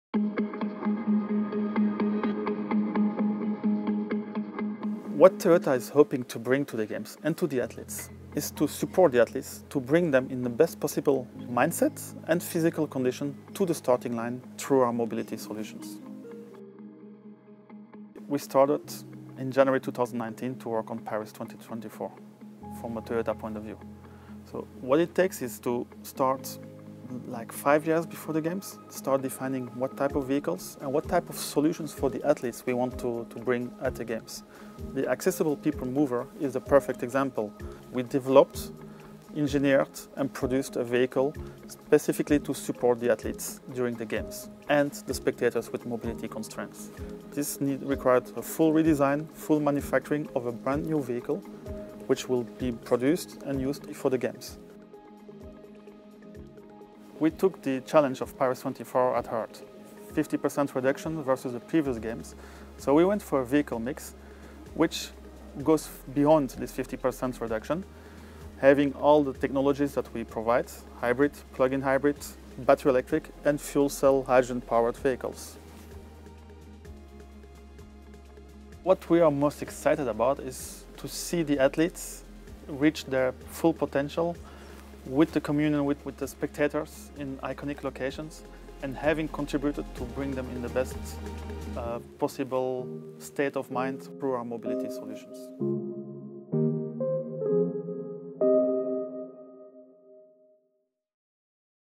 How Toyota's Innovative Mobility Solutions will support athletes at Paris 2024
As the countdown to the Olympic Games Paris 2024 continues, the Worldwide Olympic Partners are preparing to play a fundamental role in making the Games a success. In our “Road to Paris” series, Partners talk about some of the essential support they will be providing and reveal their excitement at helping to shape an unforgettable Olympic experience for fans and athletes alike. Here, we speak with Cédric Borremans, Head of the Olympics and Paralympics Department, Business Strategy Division for Toyota Europe.